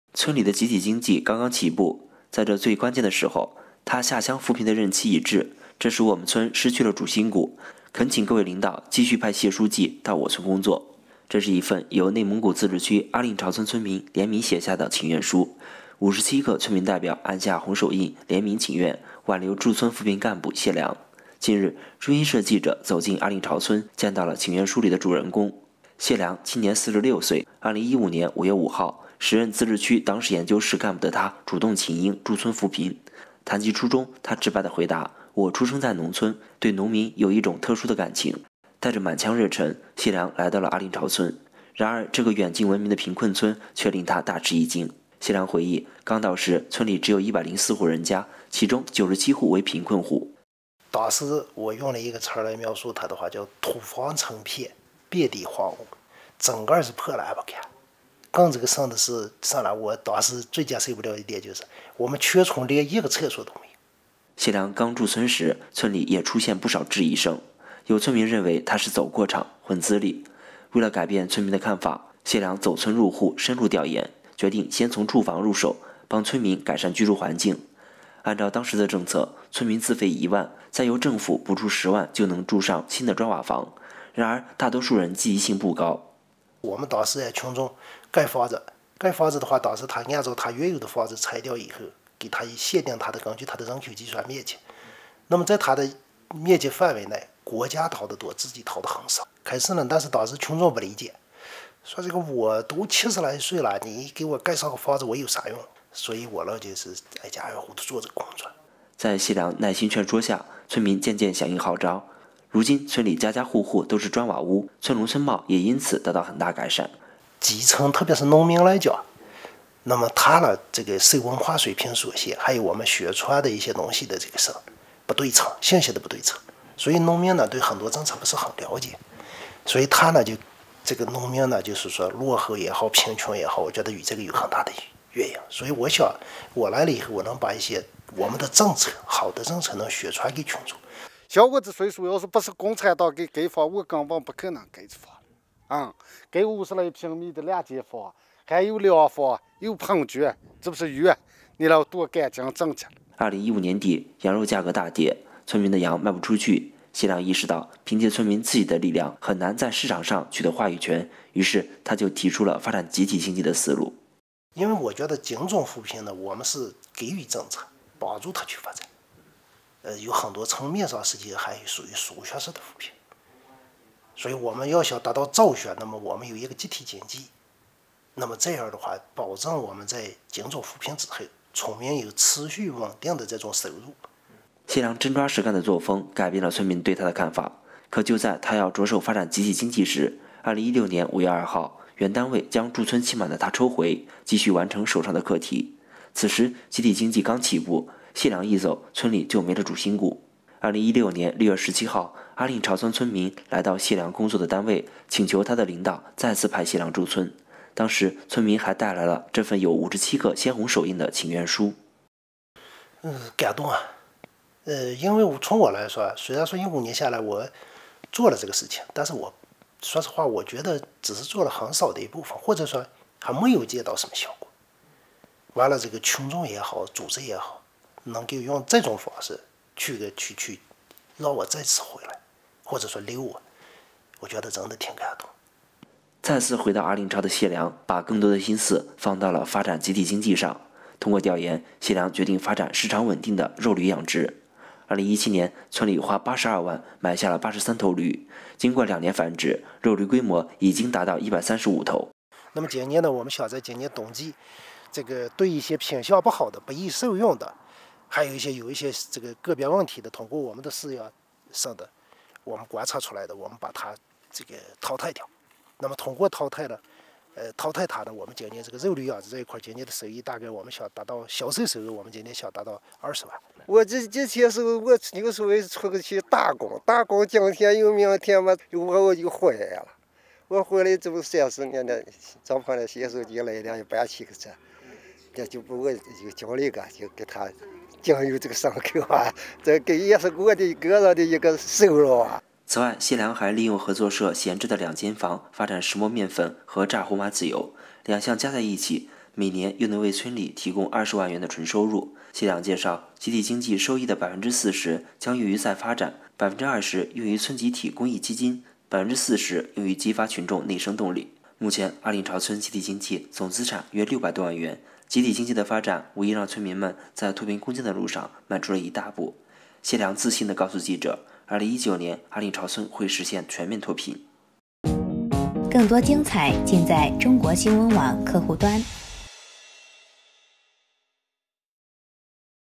【边疆党旗红】被57个红手印请回来的“第一书记”
“村里的集体经济刚刚起步”，“在这最关键的时候，他下乡扶贫的任期已至，这使我们村失去了主心骨”，“恳请各位领导继续派解书记到我村工作”。
        这是一份由内蒙古自治区阿令朝村村民联名写下的请愿书，57个村民代表按下红手印联名请愿，挽留驻村扶贫干部解良。近日，中新社记者走进阿令朝村，见到了请愿书里的主人公。